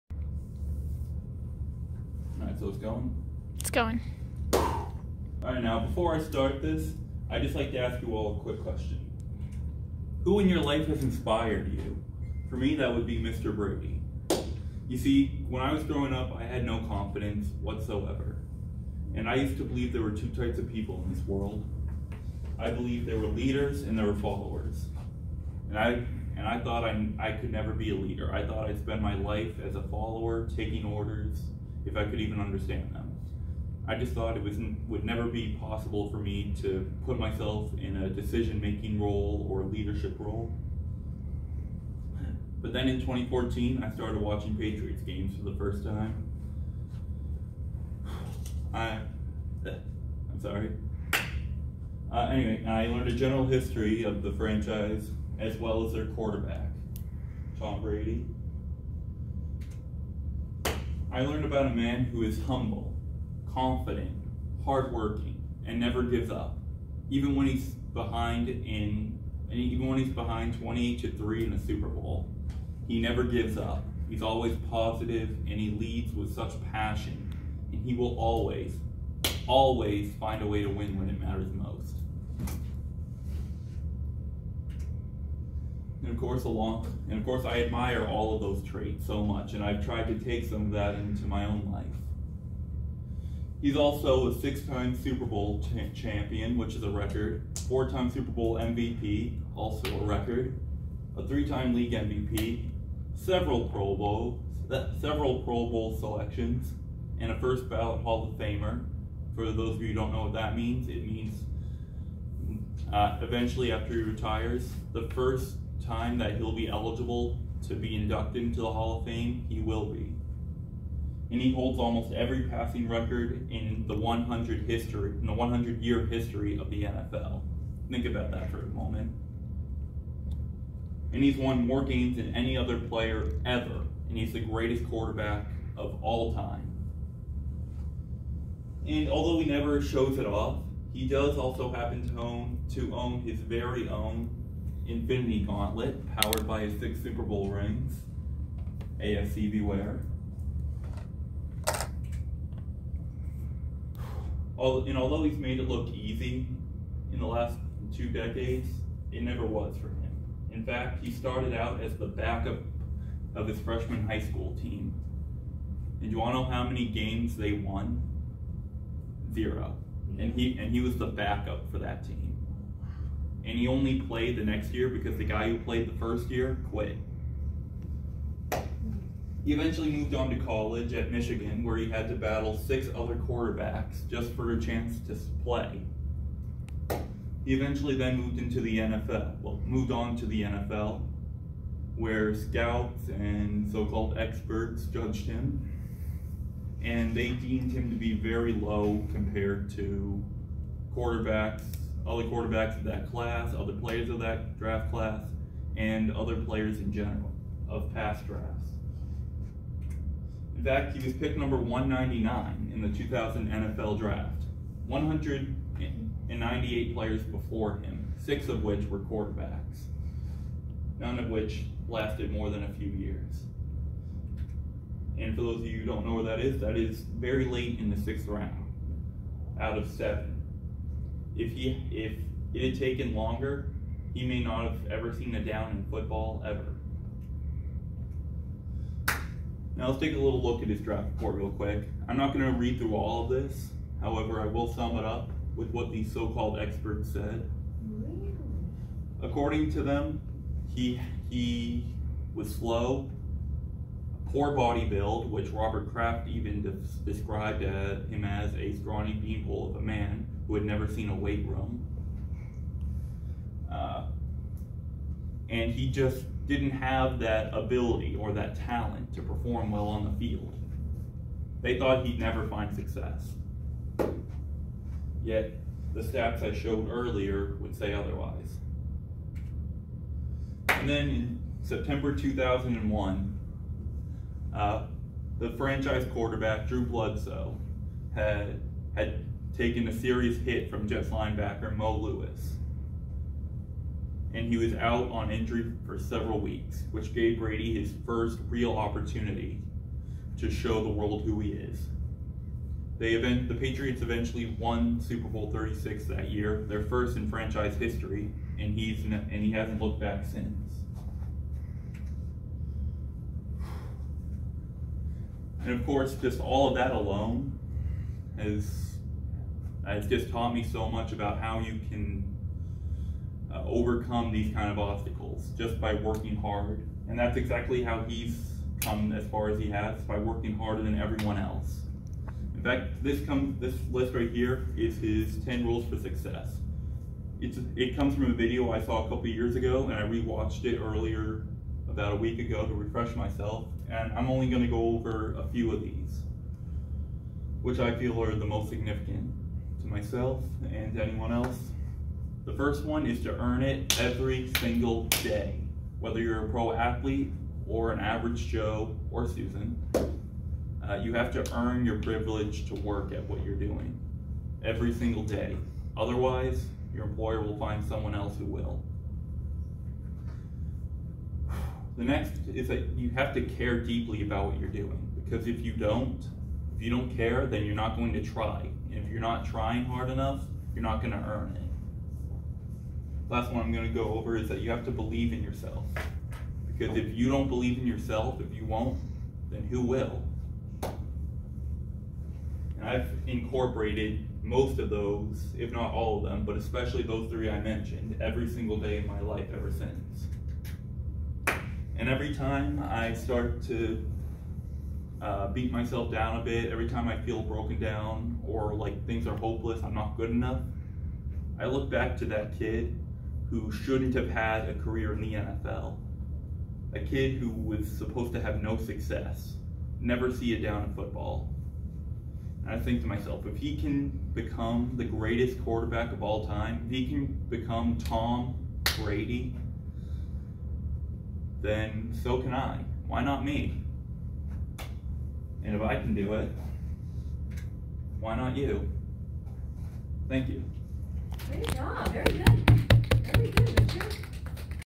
What Tom Brady has done for me
Tom Brady, if you ever somehow see this video, Thank you.
Also, in case anyone was wondering, I got an A on this school assignment